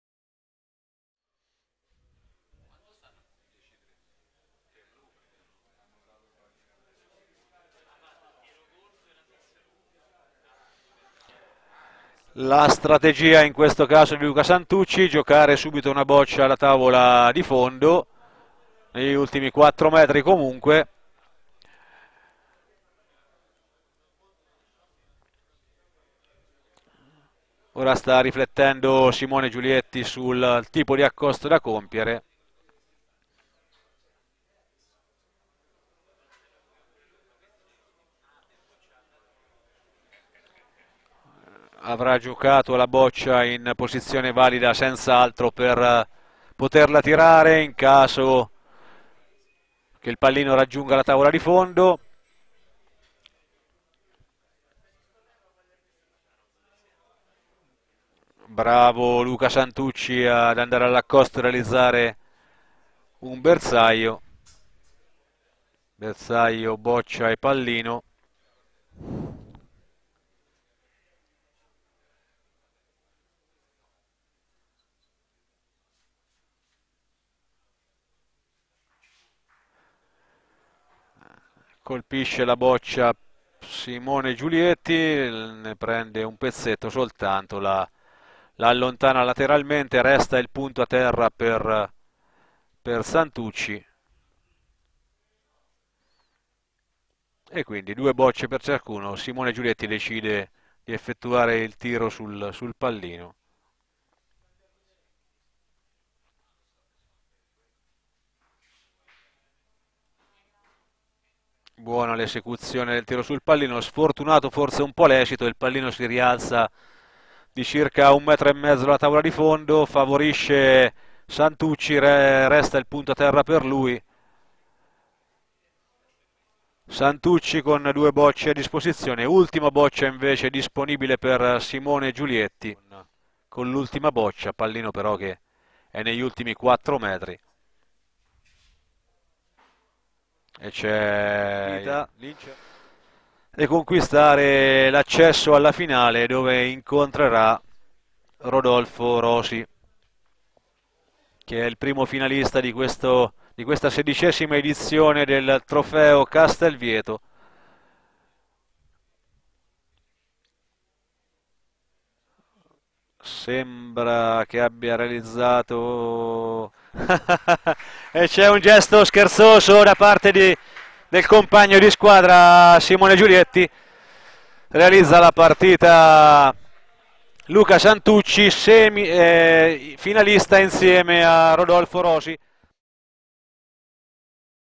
Santucci vs Giulietti. Castelvieto 2012 punteggio di 10-3
Santucci sbaglia l'acchitto del pallino e Giulietti imposta il tiro poco oltre i 14 metri. Santucci sceglie la tattica di accostare direttamente a fondo campo...
Circuito FIB Castelvieto 2012. bocce sezione raffa.